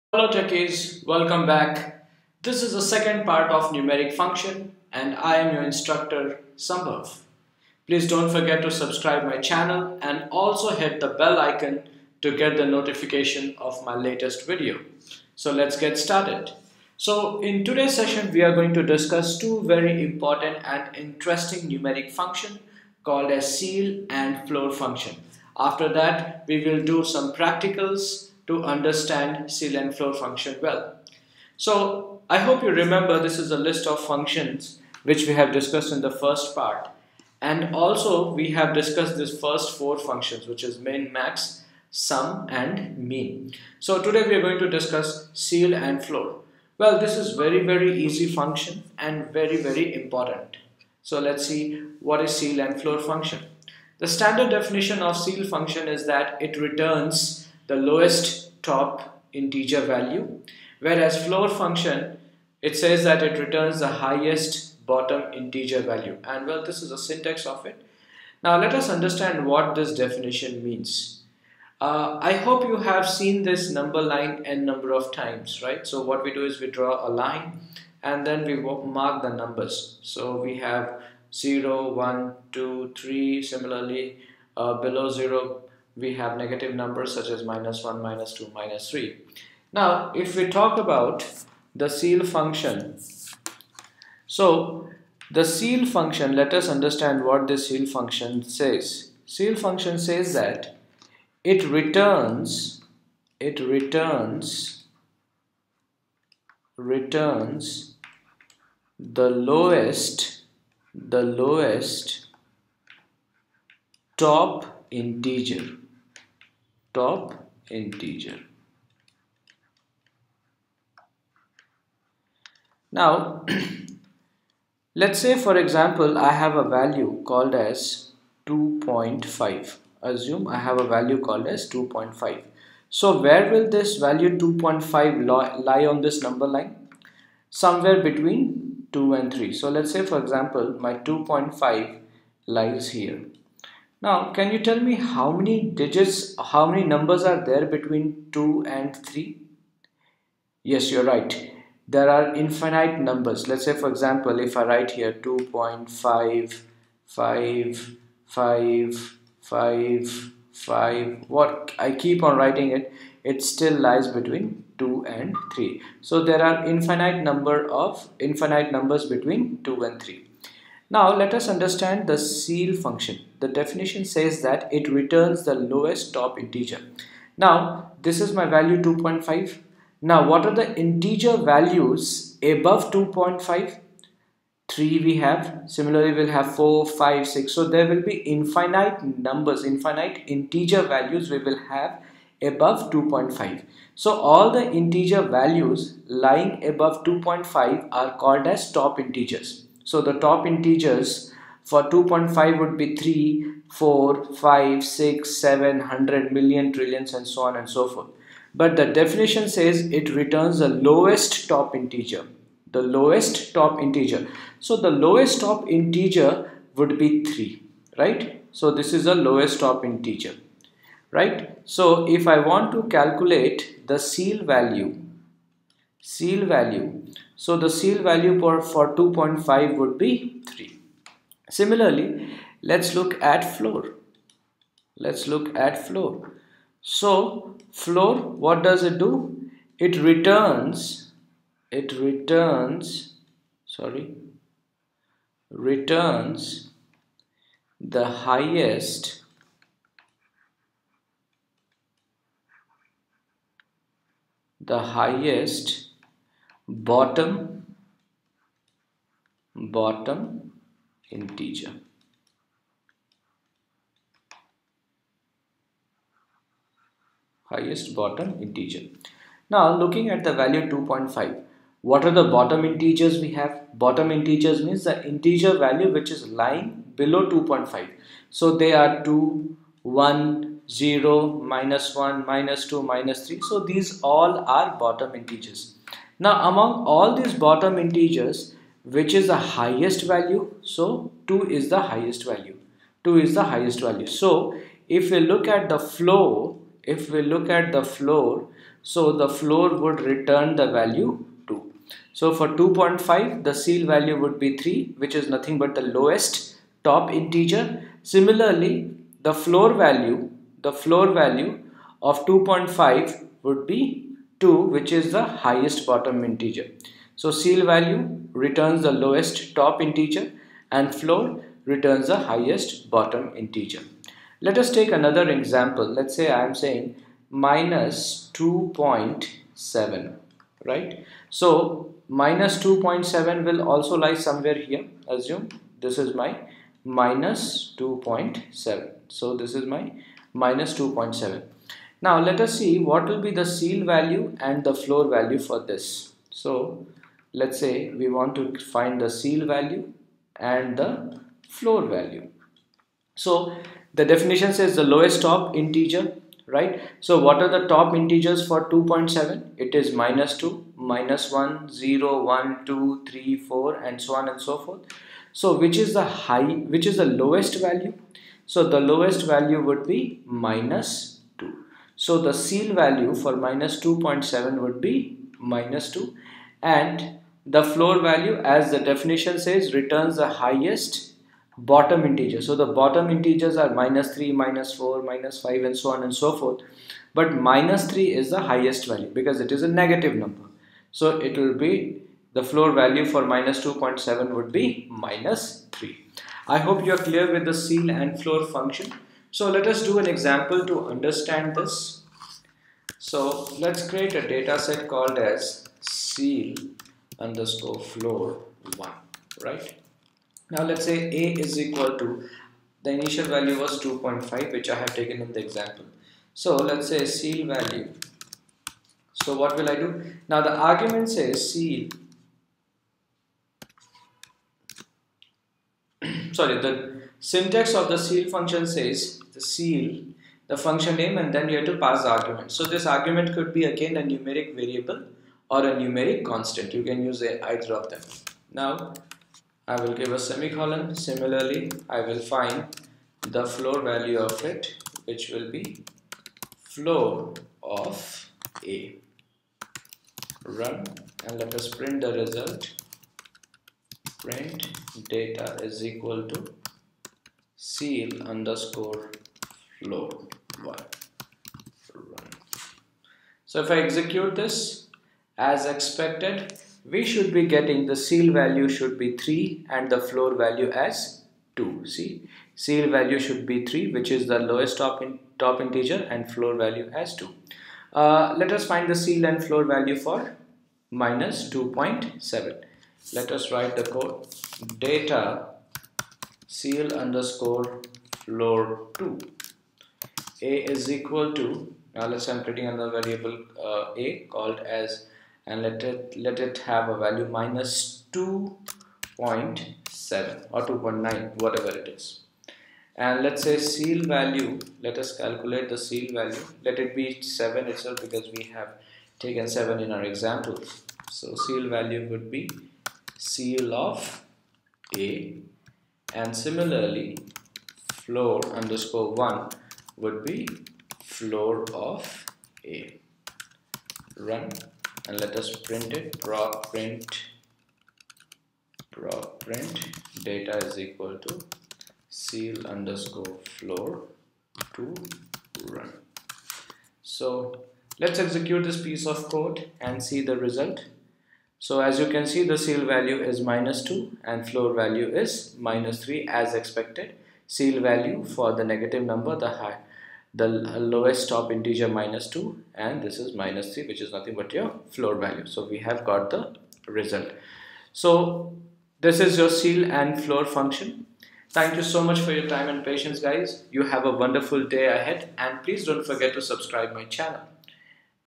SAS Numeric Function - PART 2 (Base SAS Programming)
Hello Techies,
In this video we have discussed SAS numeric functions.
You will learn the following :
Ceil Function in SAS
Floor Function in SAS
Practical Examples of Ceil and Floor Function in SAS.